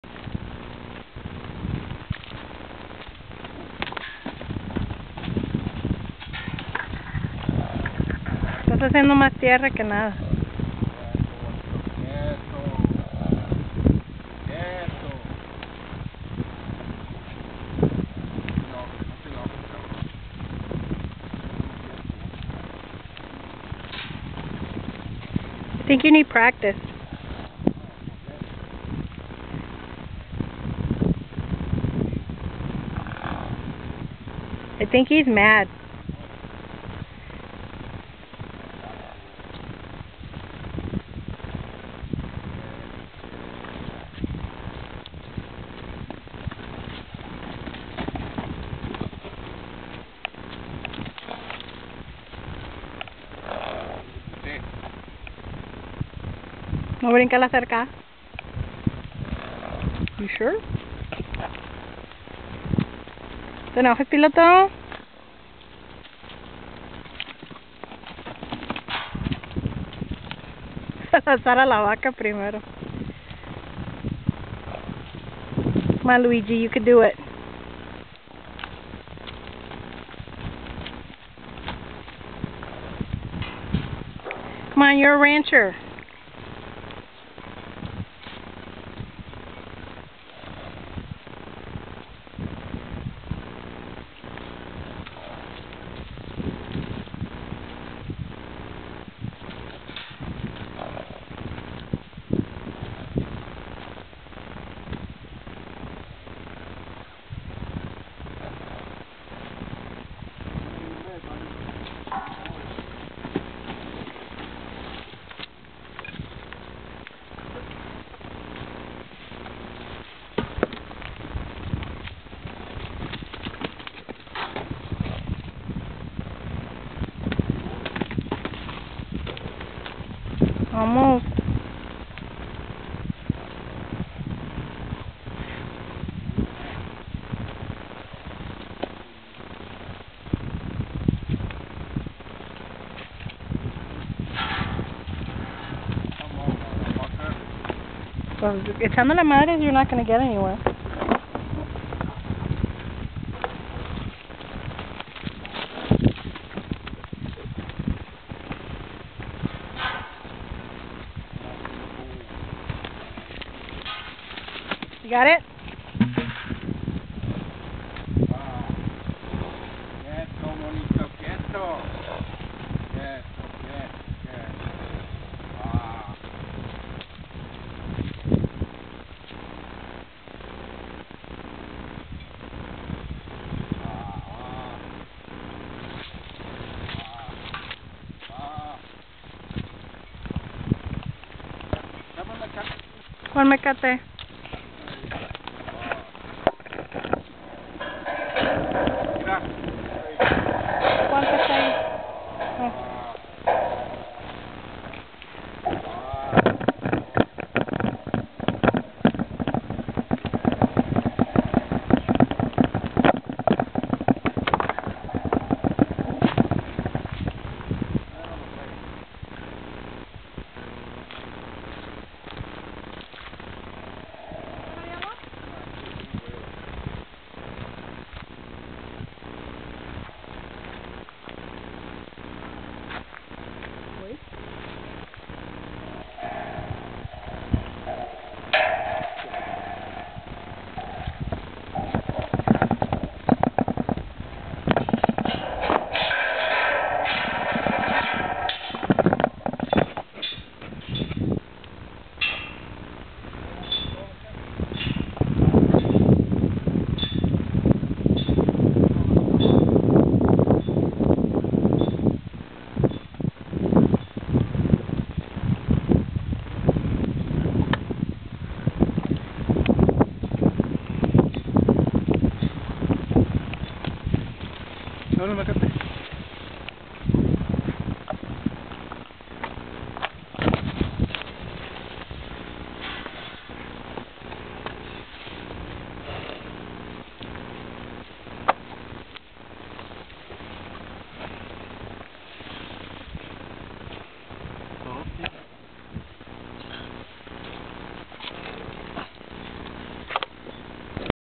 Piloto 1
Luis attempting to catch his horse "Piloto".